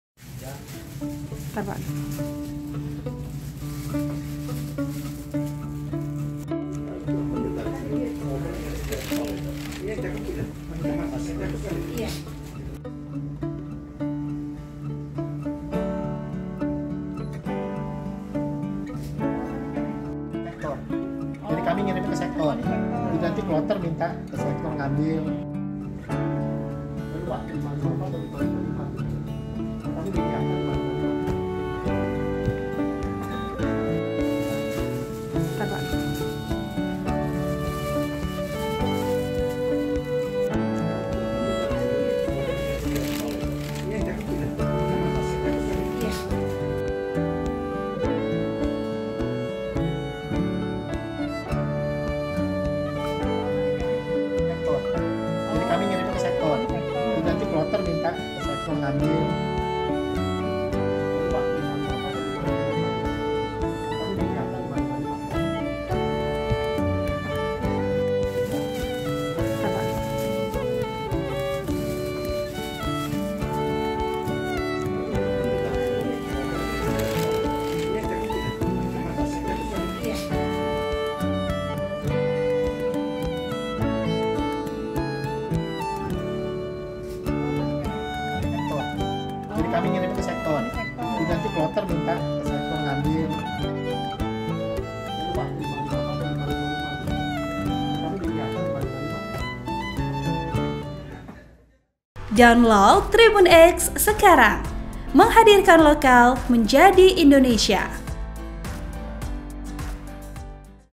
Pastikan Jemaah Bisa Ibadah dengan Tenang, 555 Paket Obat Disiapkan Jelang Puncak Haji di Armuzna
TRIBUN-VIDEO.COM - Kesehatan Haji Indonesia (KKHI) menjamin stok obat untuk jemaah haji saat pelaksanaan puncak haji di Arafah, Mina dan Muzdalifah (Armuzna) aman,

Ahadi Wahyu Hidayat, Koordinator Obat dan Perbekalan Kesehatan KKHI Makkah, mengatakan KKHI menyatakan 555 paket obat akan dibagikan ke petugas kesehatan di setiap kloter.

Juga ada paket obat untuk jemaah yang akan disafariwukufkan.

Editor Video: Ilham Bintang Anugerah
Uploader: Aditya Wisnu Wardana

Program: Haji 2024